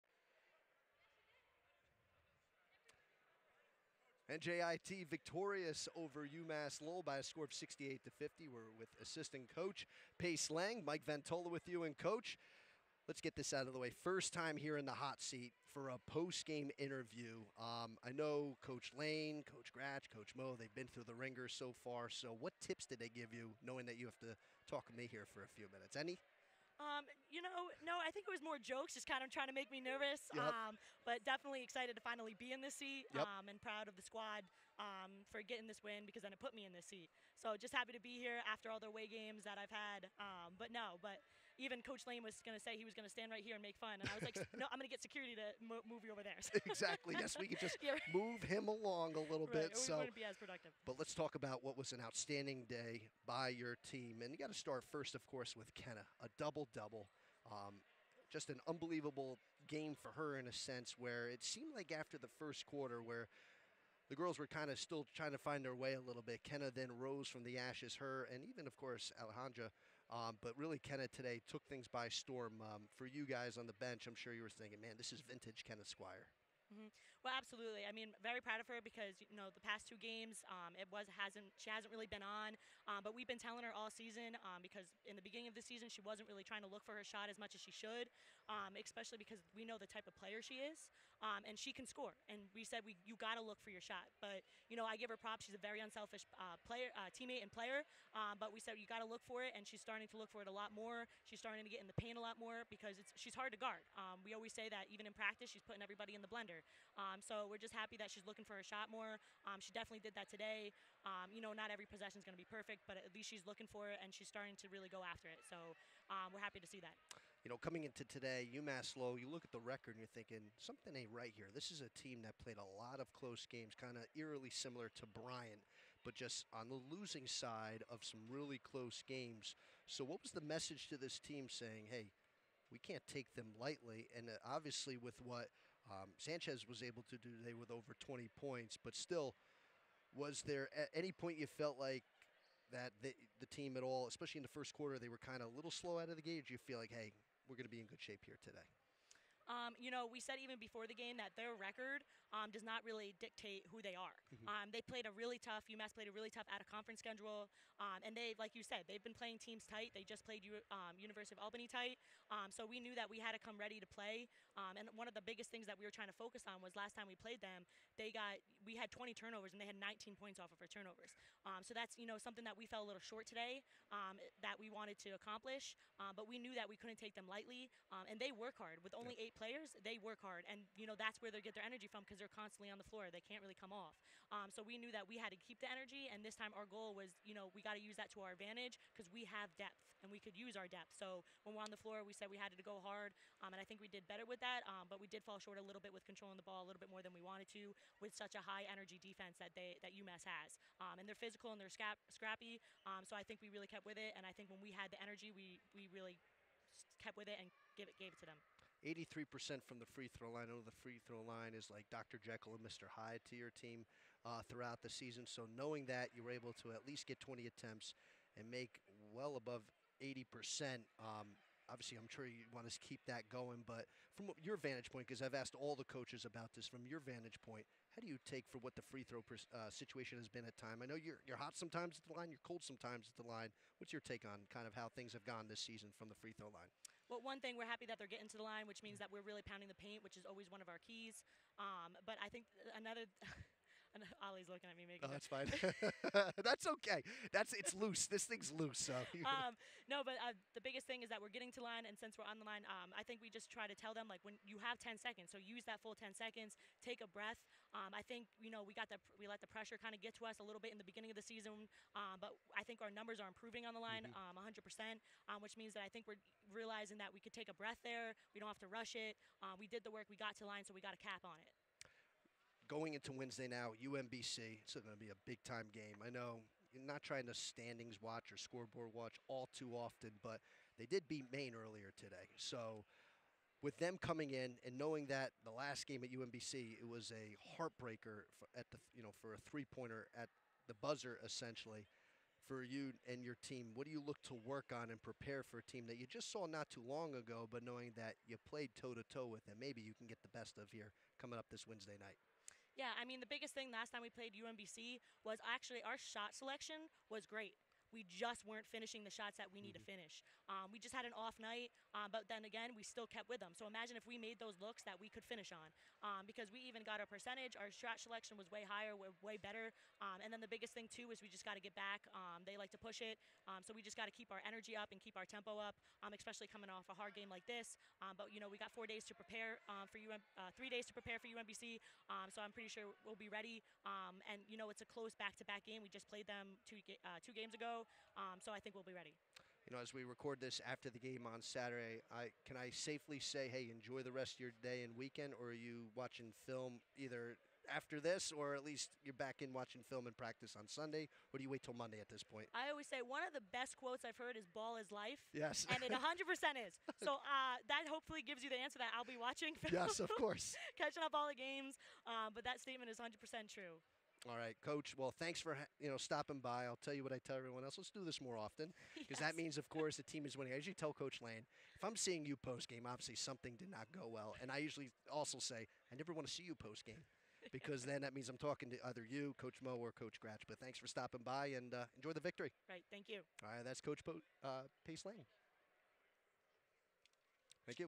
Post Game Wrap vs. UMass Lowell with Assistant Coach Payce Lange | 2.4.23
NJIT senior Kenna Squier registered her third career double-double, netting a game-best 22 points and 10 rebounds, leading the Highlanders to a 68-50 home victory over UMass Lowell Saturday afternoon.